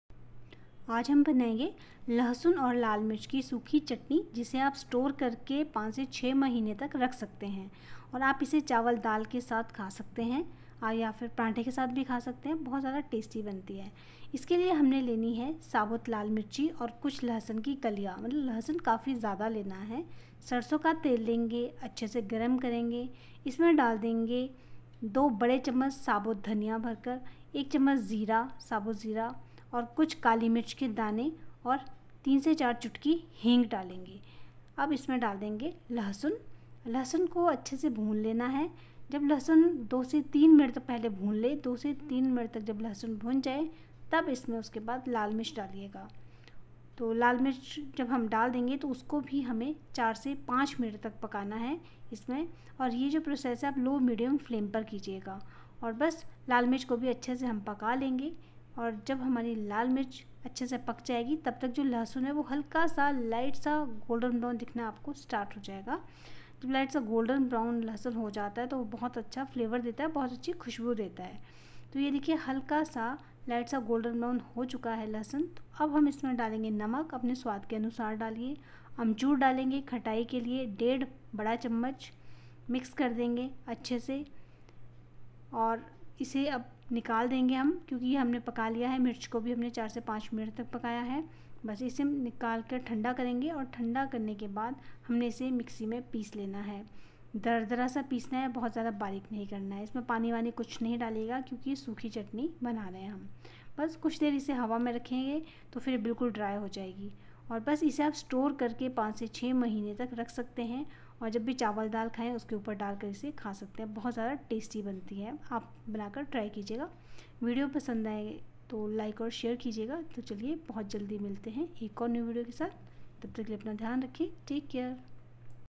लहसुन और लाल मिर्च कि सूखी चटनी ऐसे बनाएँगे तो दो की जगह चार रोटी खाएँगे-Garlic Chutney-Lehsun chtny
Video Copyrights – Cooking with Rekha(verified)
Thumbnail Copyrights – Cooking with Rekha(verified)
Audio Copyrights – Cooking with Rekha(verified)